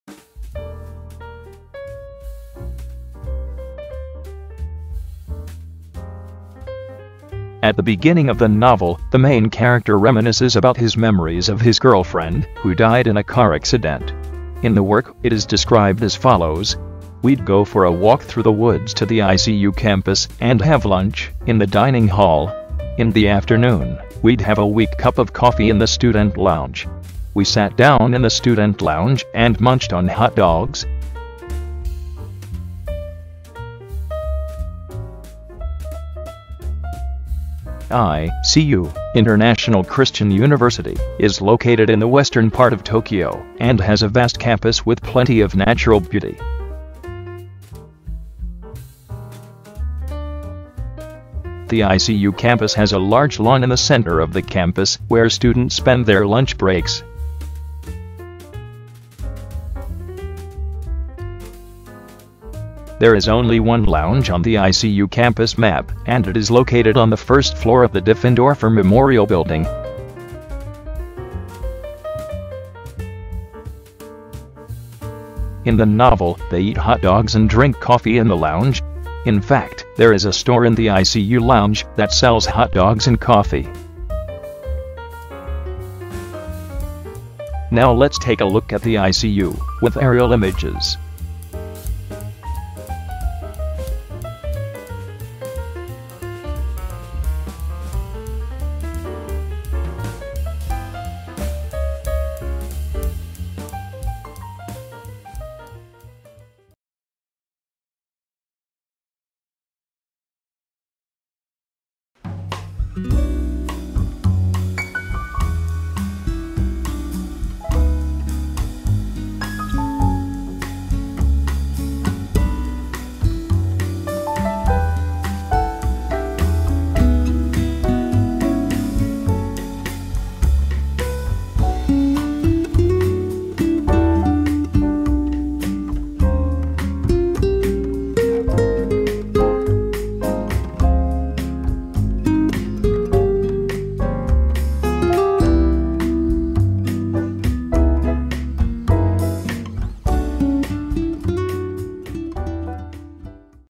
ICU from "A Wild Sheep Chase" by Haruki Murakami
We made a video of the scene in the ICU at the beginning of "A Wild Sheep Chase" by Haruki Murakami.

- Chapters -
0:00 Main chapter
1:26 Aerial video
1:48 Photos related to the work